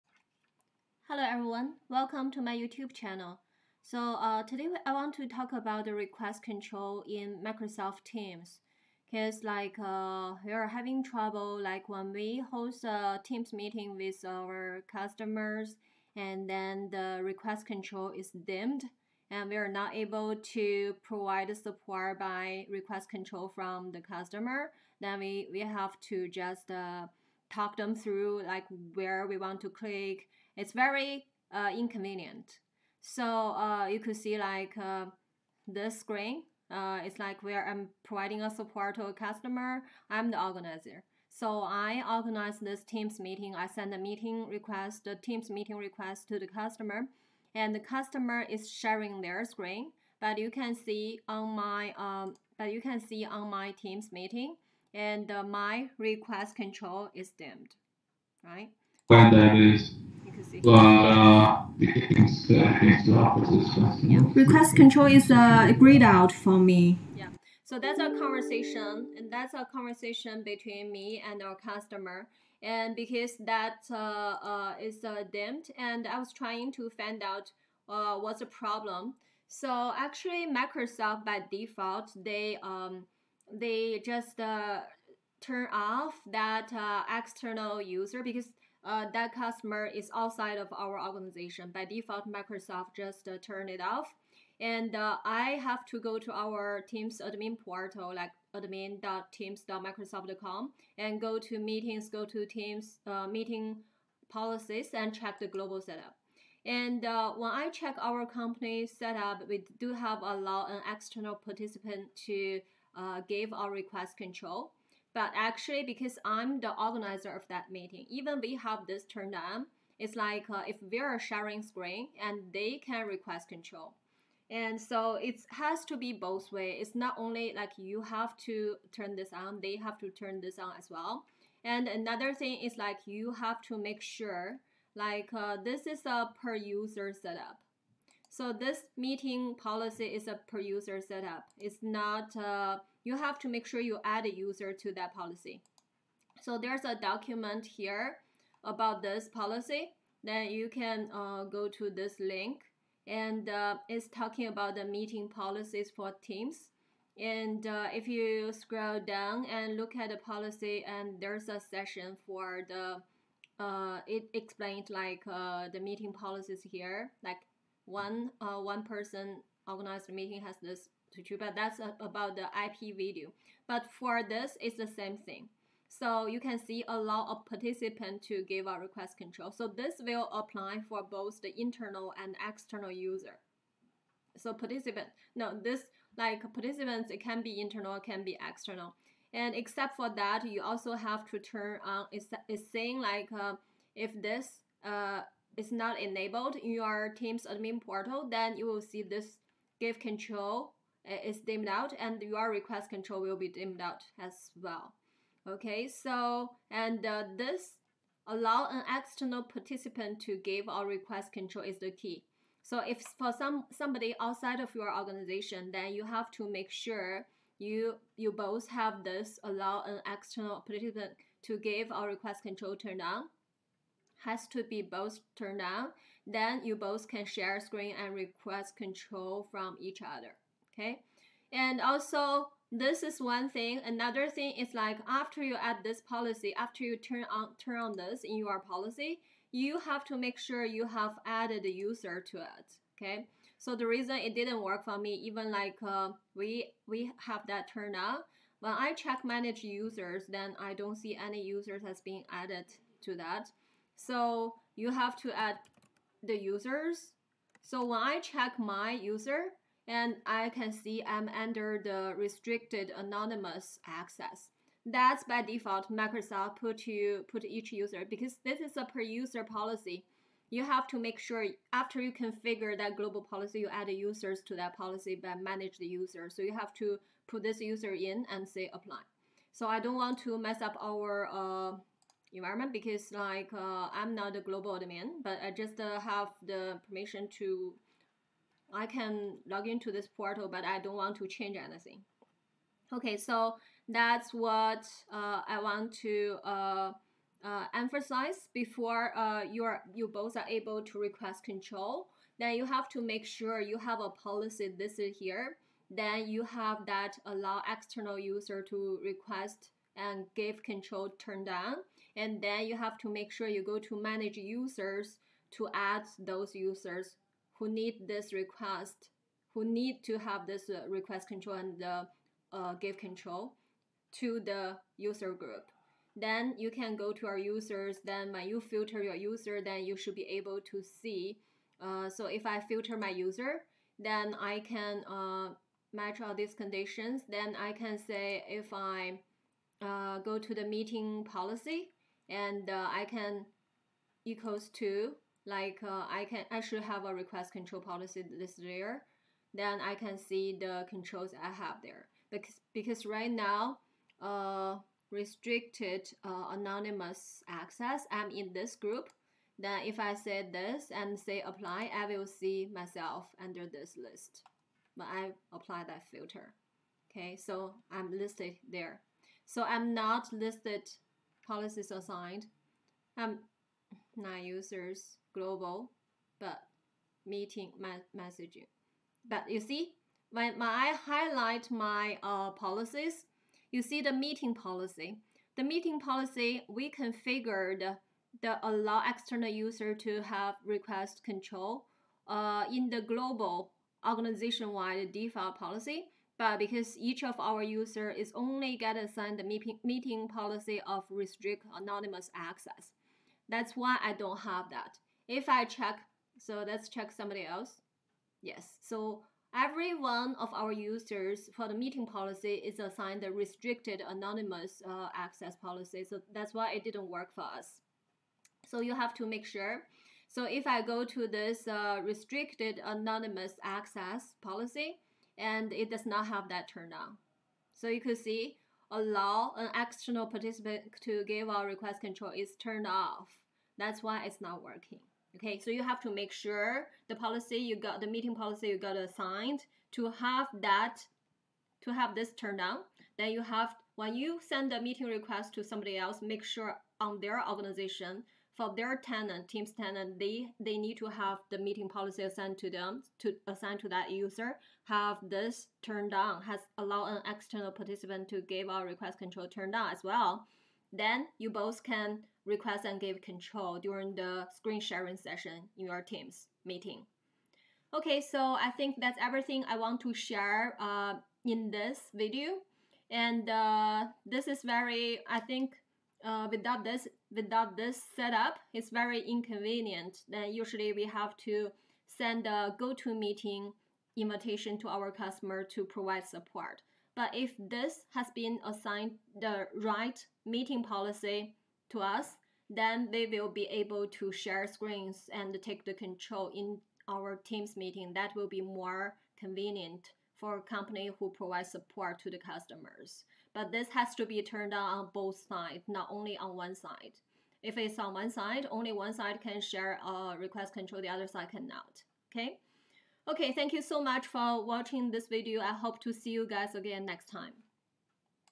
Request and Give Control in Teams Meeting
This video demonstrated how to configure your Microsoft Teams to make the request and give control available for external users. This setting is disabled by default by Microsoft and because it's per user policy. You need to enable it in your Teams Meeting Policy and add users to that Meeting Policy.